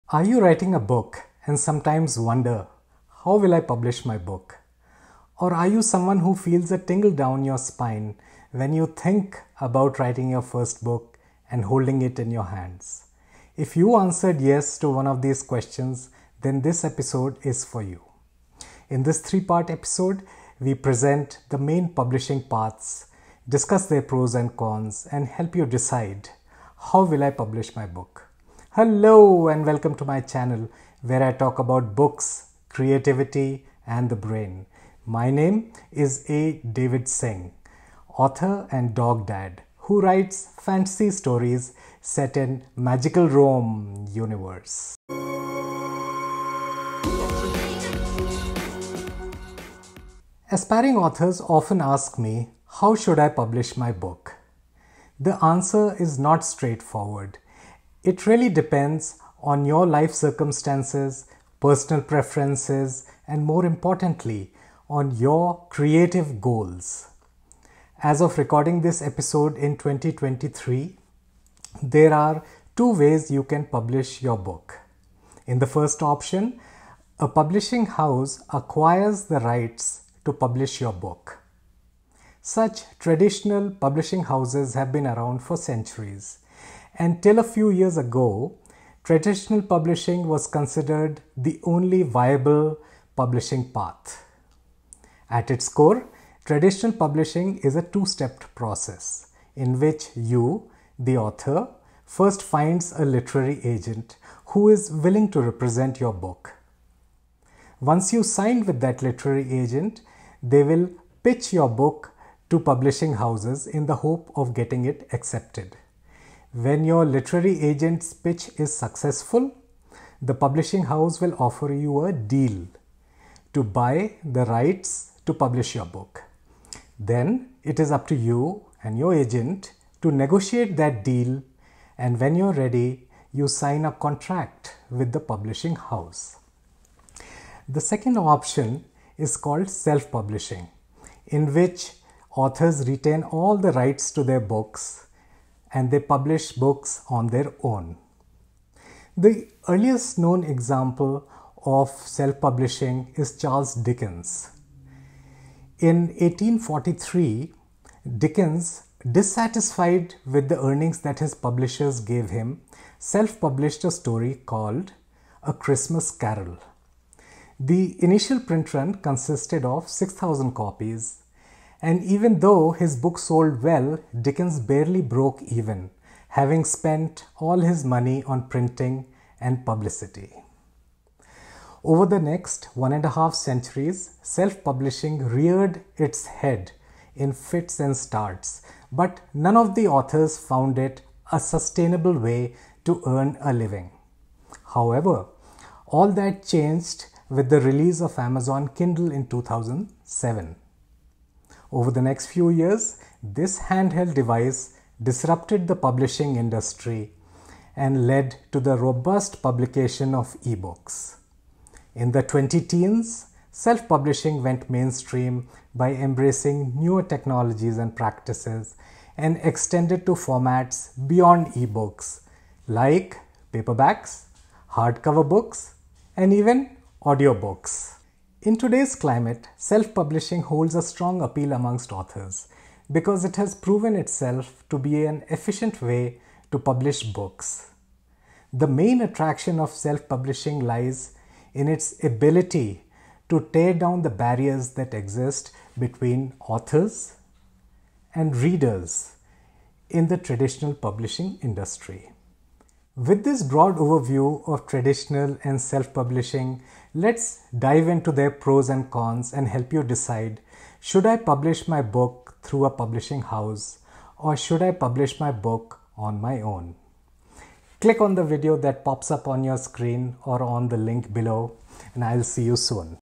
How to Publish Your Book | Self Publishing vs Traditional Publishing — Part 1
Discover if SELF PUBLISHING or TRADITIONAL PUBLISHING is the right choice to publish your book.

This video is Part 1 of “How to Publish Your Book: Self Publishing vs Traditional Publishing”.

In this video, we talk about the differences between SELF PUBLISHING and TRADITIONAL PUBLISHING.

Part 3 (why should you SELF PUBLISH, why you should TRADITIONALLY PUBLISH, and TWO FACTORS common to both self publishing & traditional publishing)

Free PDF - Top 10 book recommendations for creating Captivating Characters and Nail-biting Plots

- Dive into the fun "Which Magical Rome Character Are You?" Quiz

Timestamps:
00:00 Self publishing vs Traditional publishing
01:08 Traditional publishing
02:17 Self publishing
04:13 Watch Part 2